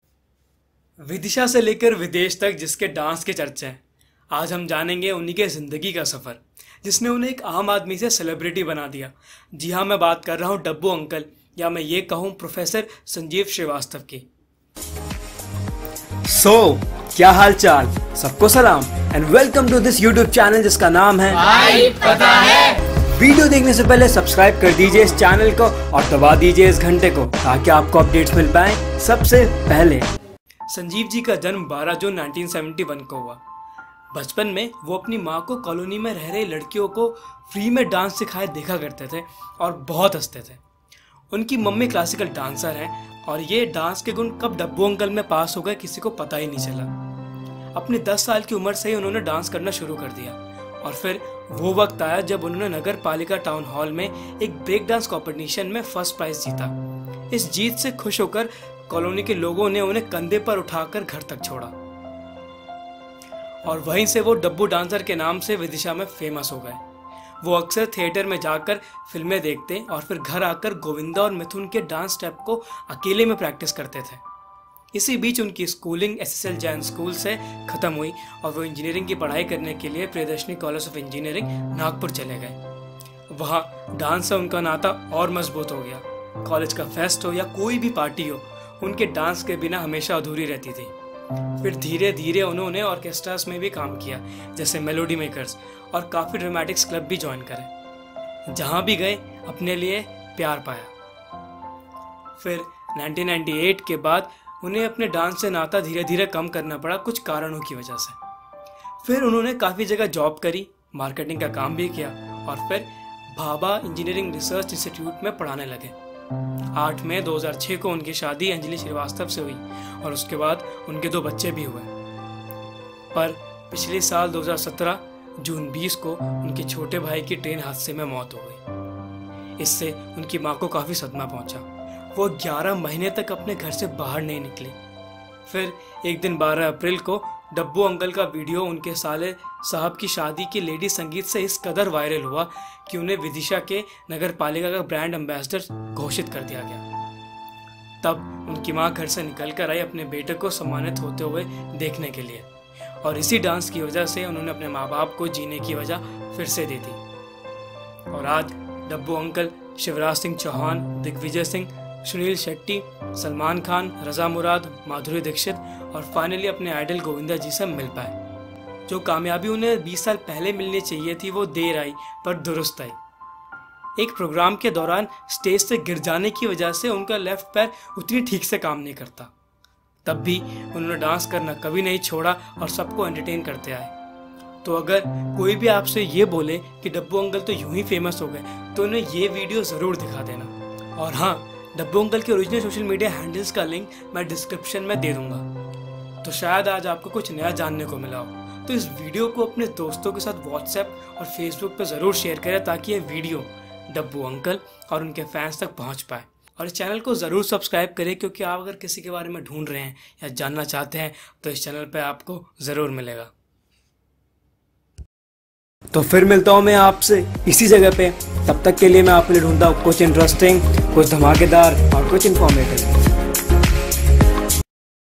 Dabbu Uncle Complete Biography | Family | Sanjeev Srivastava | Viral Dancer
vidhisha se leke videsh tk jiske dance ke charche hai,aaj hum jaanenge unhi ke zindagi ka safar, jisne unhE eek aam aadmi se celebrity bna dia. mai baat kr rha hu dabbu uncle ya mai ye kahu professor Sanjeev shrivastava

Sanjeev ji ka janam 12 june 1971 ko hua. Bachpan me voh wapni maa ko colony me reh ri  ladkio ko free me dance sikhaye  dekha krte the aur bht haste the.

NOTE: All content used is copyright to Bhai Pata Hai,